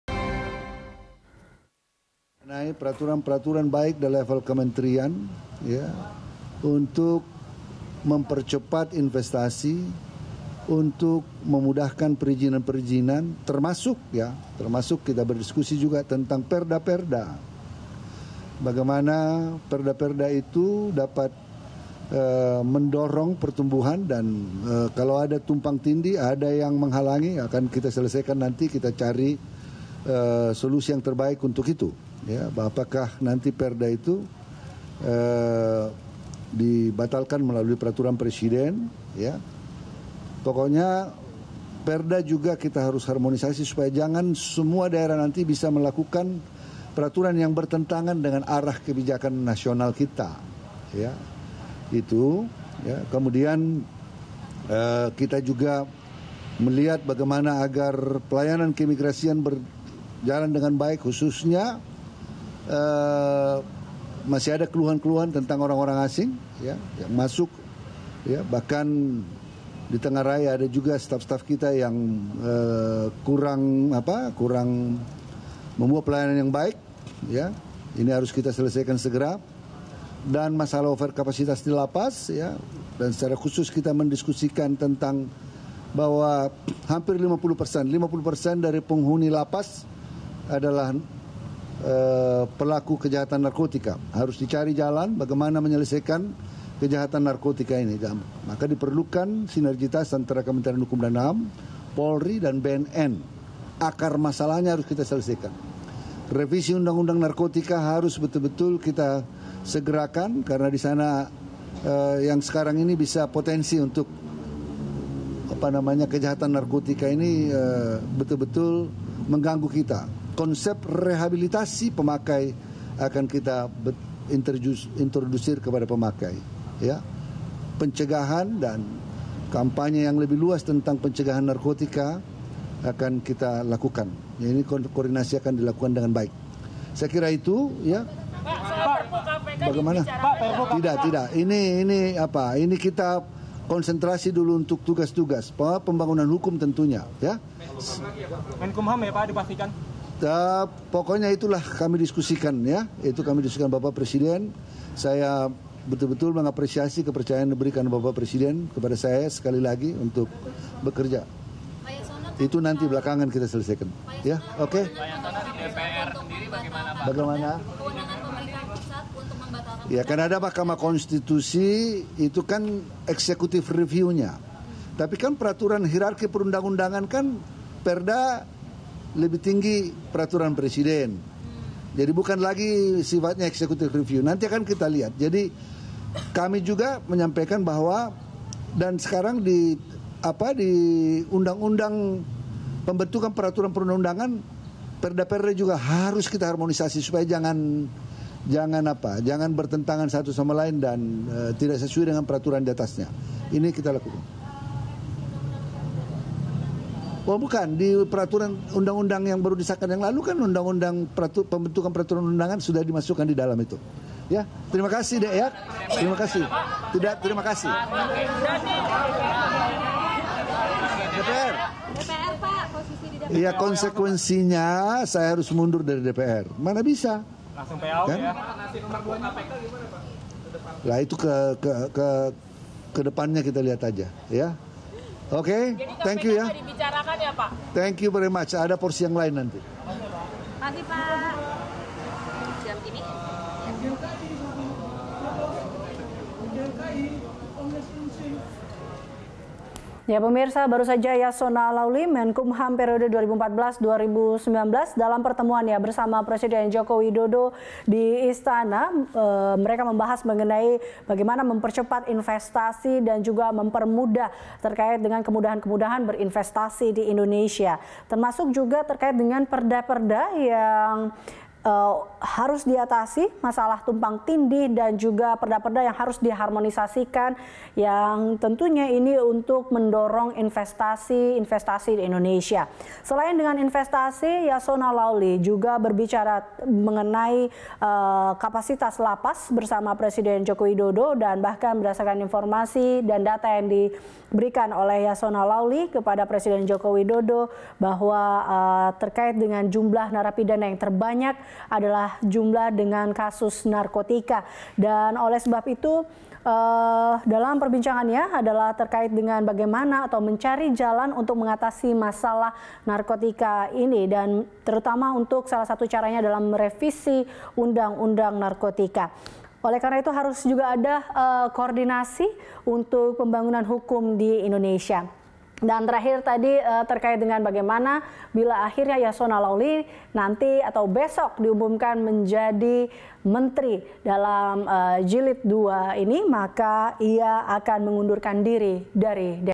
Terpilih Jadi Menteri, Yasonna Siap Mundur dari DPR
Yasonna Laoly memenuhi panggilan Presiden Jokowi di Istana Kepresidenan, Jakarta, Selasa (22/10/2019). Bila dipercaya kembali menjadi menteri, Yasonna siap mundur dari DPR.

Facebook.com/BeritaSatu
Youtube.com/BeritaSatu